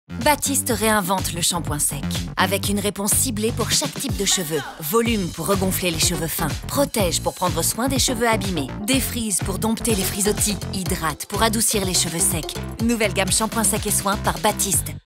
Pub Batiste Shampooing sec et soin 2019-15 sec
Batiste réinvente le shampooing sec avec une réponse ciblée pour chaque type de cheveux! Découvrez sans tarder la nouvelle gamme de shampooings secs & soin Volume, Hydrate, Protege, De-Frizz,